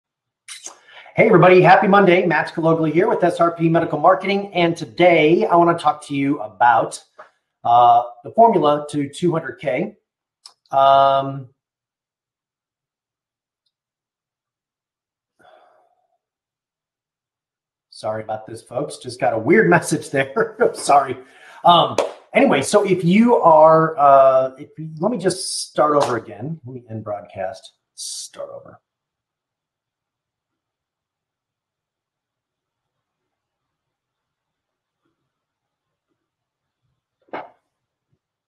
Why Patients Don't Buy
Still not doing $200,000 per month in your clinic? Let me share with you a reason why that is happening.

This morning I was going over a training for my clinics. I'm always looking for a slight edge, something to give our team.

Back in 2016, we were struggling converting leads into patients. My staff started telling me that the leads weren't any good. Which was funny because the month prior to the same lead sources gave us a record month!

I knew it wasn't the leads, it was how we were approaching the leads. You see my team after a record $900k month, was struggling to hit our basic goal.

Here is what I shared with them. If you'd like this formula just type the words 'Formula' below.